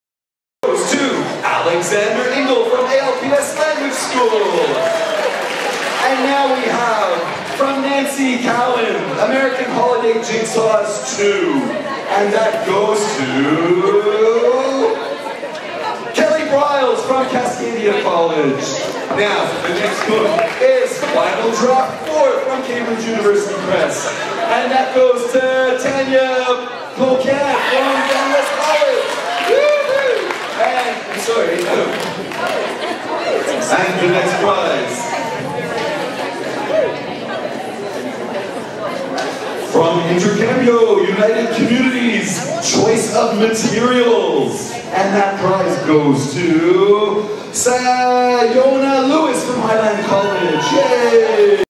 The TriTESOL Door Prize is Right with Scott Douglas
Our one and only Scott Douglas handing out publisher prizes at Tri-TESOL 2015